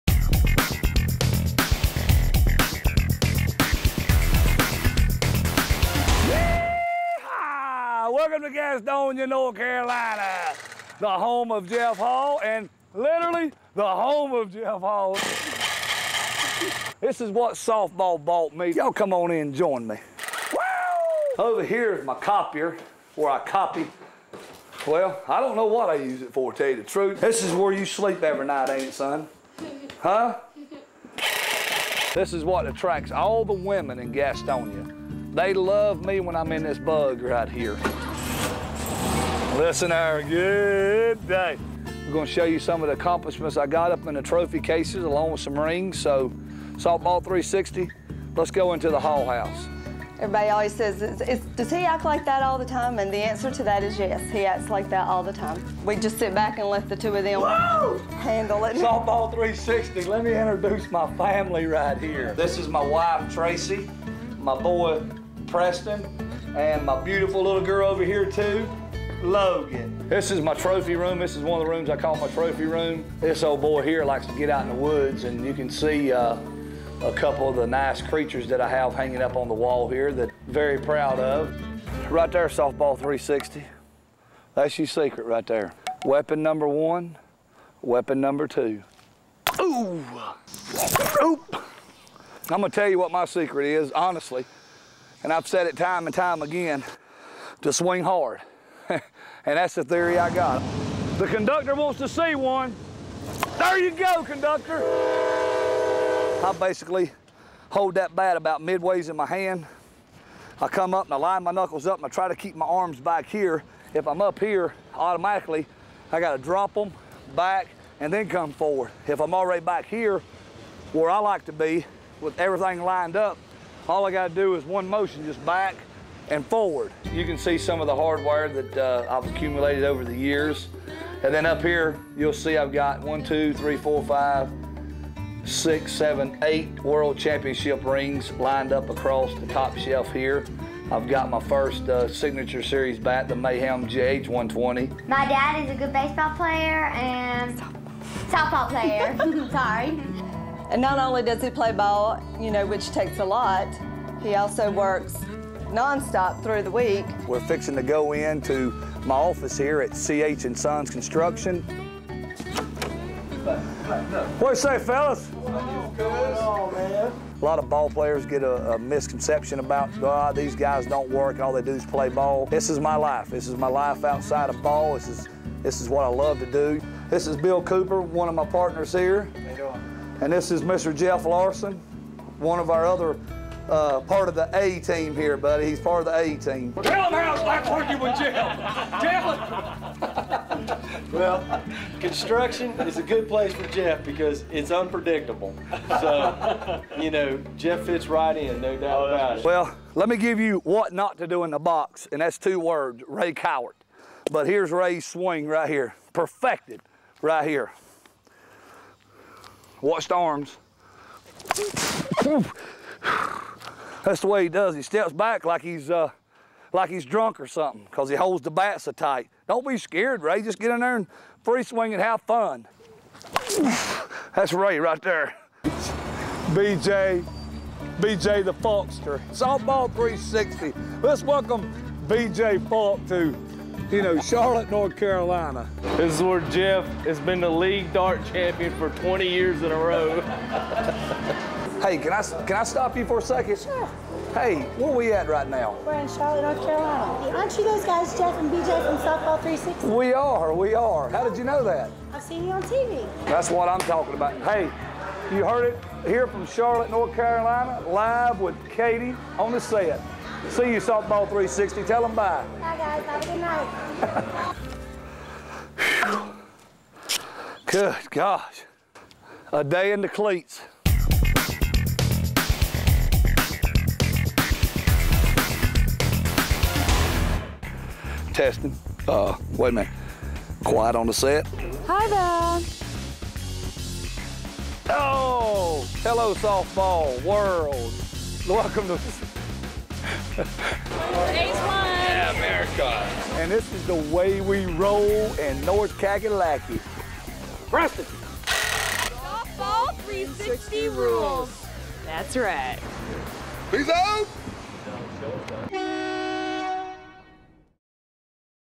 Softball 360: Jeff Hall Spotlight
I produced, shot, and edited this feature on professional softball player Jeff Hall.